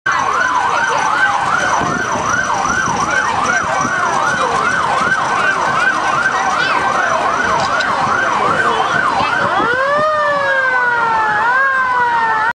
Veterans at Hahira Honeybee Festival
Hahira Honeybee Festival, Hahira, Lowndes County, Georgia, 8 October 2011.
Pictures and Videos by John S. Quarterman for LAKE, the Lowndes Area Knowledge E
xchange.